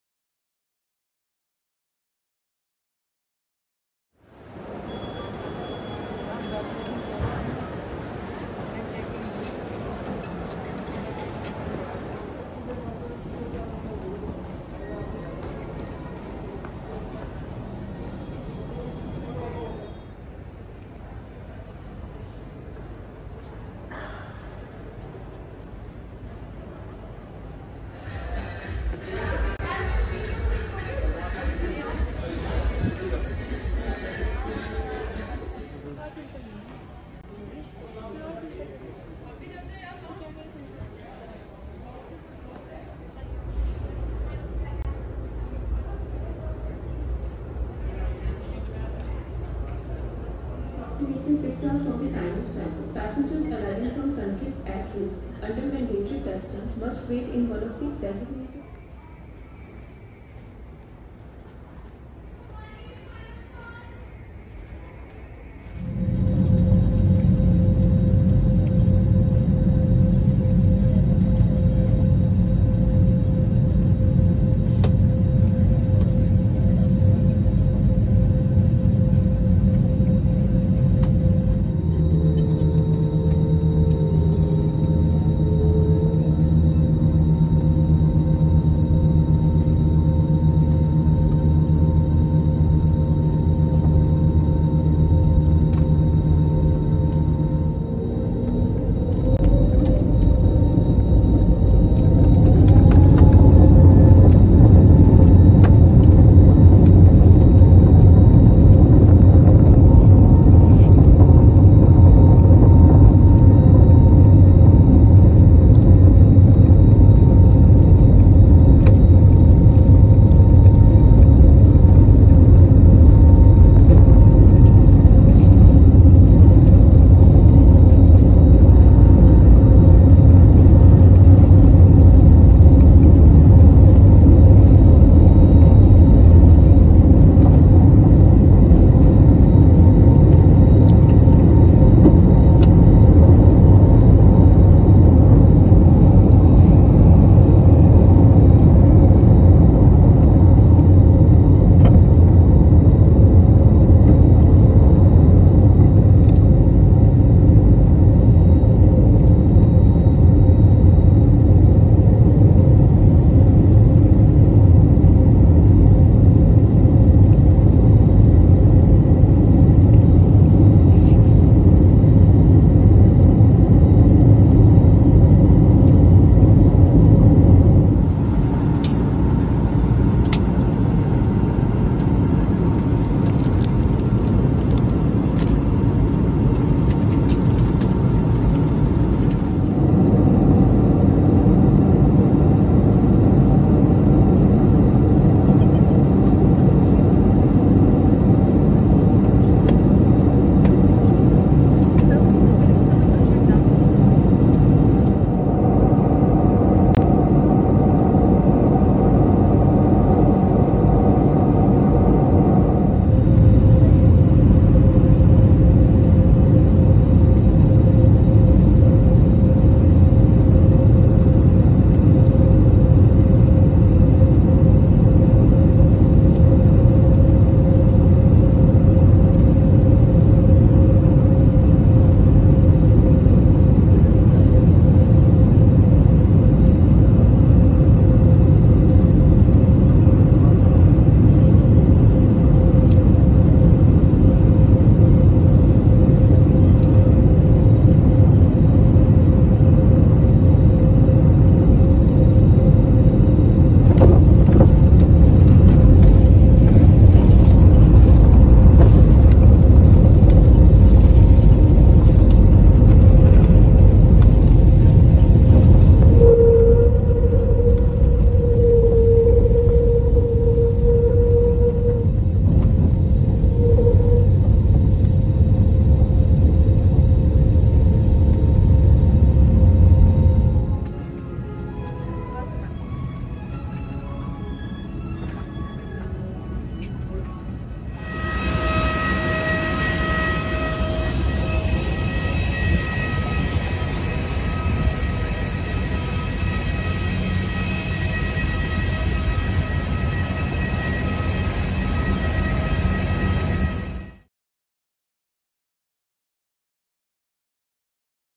Delhi to Male Flight
A short video of my flight from Delhi to Male in January 2022.